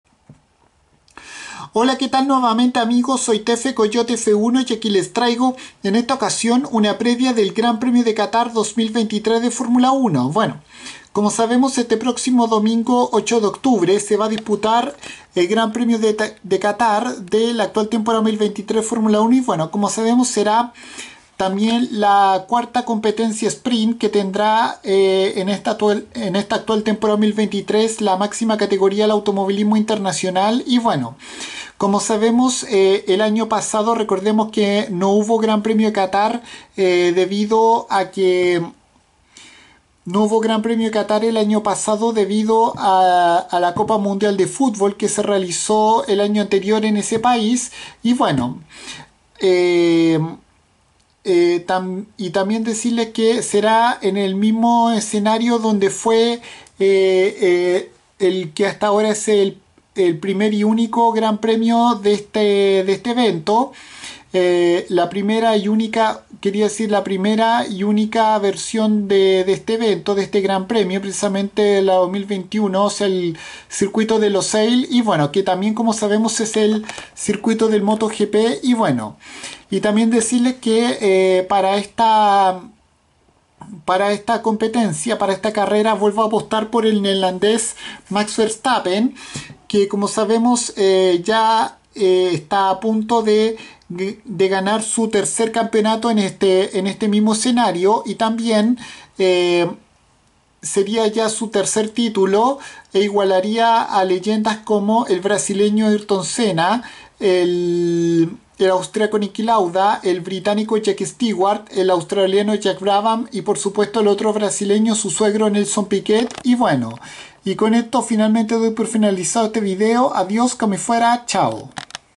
Previa GP de Catar 2023 de F1
Una breve previa de lo que será el 2do Gran Premio de Catar en el Circuito de Losail de la Temporada 2023 de la Fórmula 1(cuya versión del año 2022 no se hizo por la Copa Mundial de Futbol de FIFA de ese año)

Para esta competencia vuelvo a apostar por el neerlandés oriundo de Hasselt Bélgica Max Emilian Verstappen para que gane aquí por fin su ansiado 3er título y además por Fernando "Magic" Alonso para que obtenga su ansiada victoria "33"